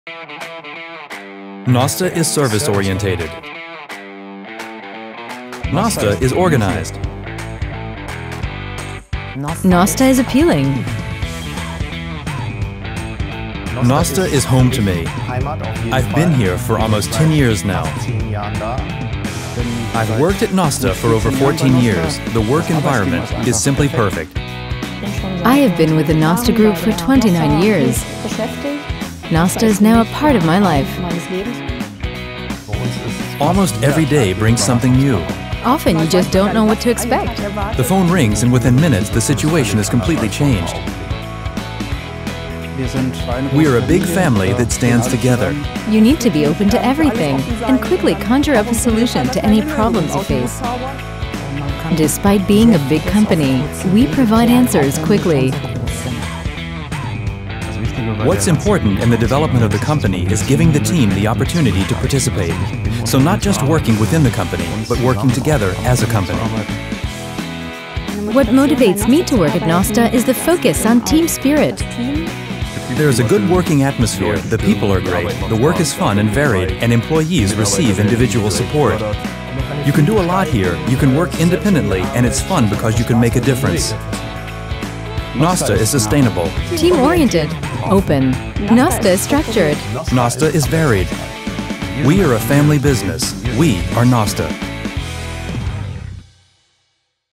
NOSTA Employer Branding - EN
At the NOSTA Group, people make the difference. This impressively shows the new image film of our company. Through concise interview statements of our employees from different areas it becomes clear what makes our company: NOSTA is more than an employer, NOSTA is family!